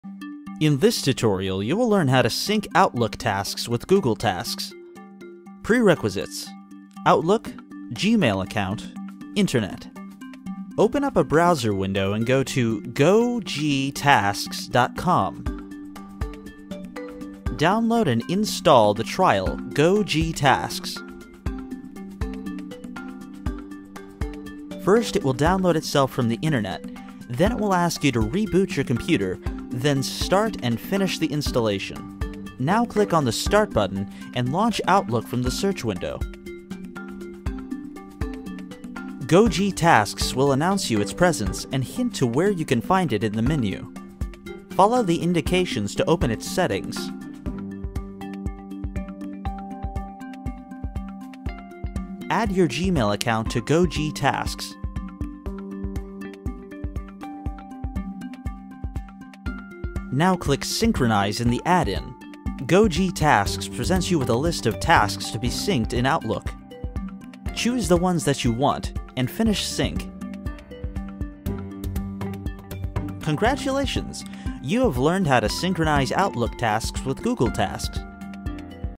How to Sync Outlook Tasks with Google Tasks
In this tutorial you will learn to sync outlook tasks with google tasks.

Step 2: Download and install the trial Gogtasks.

First it will download itself from internet, then it will ask you to reboot your computer, then start and finish installation.

Step 3: Click the Start button and launch outlook from the search window.

Step 4.: Gogtasks will announce you its presence and hint to where you can find it in the menu. Follow the indications to open its Settings.

Step 5.: Add your gmail account to Gogtasks.

Step 6: Click Synchronize in the Add-in.

Step 7.: . Gogtasks presents you with a list of tasks to be synced in Outlook. Choose the ones that you want, and finish sync.

Result:  Congratulations, you have learned how to synchronize outlook tasks with google tasks.